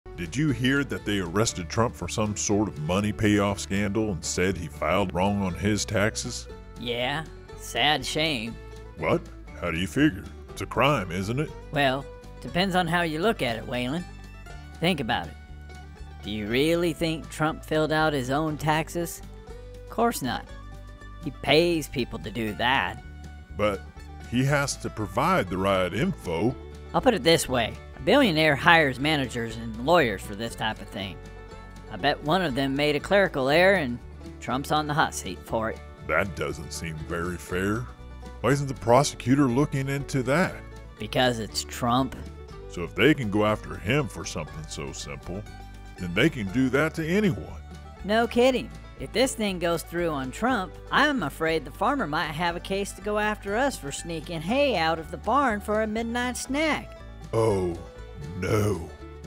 Trump Arrest - Willie Explains His Take
Willie and Waylon are two comedic farm goats and discuss their take on Trump's arrest.

Hey, the farmer only feeds us so often and it takes a lot of energy to make these videos.